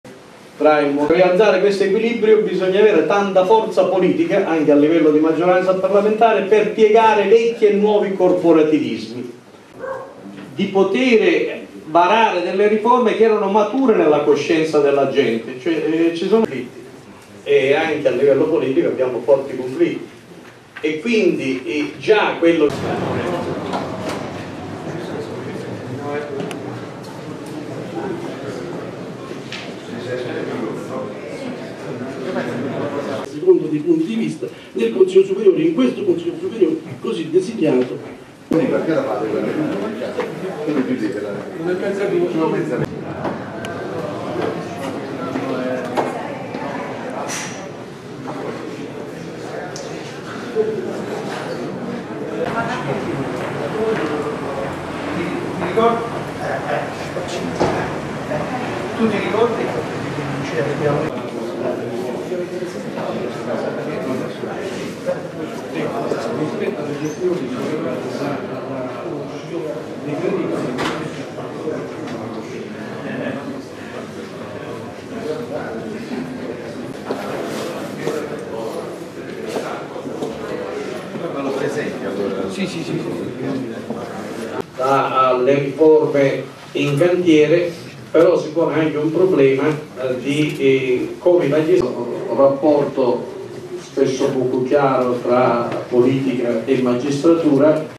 IL LUNGO CAMMINO DELLA GIUSTIZIA - CONVEGNO A NAPOLI
(Agenzia VISTA) - Napoli, 16 Gennaio 2016 - Altrimondi. Il convegno "Il lungo cammino della Giustizia" con la parlamentare PD Rostan e l'europarlamentare Gianni Pittella.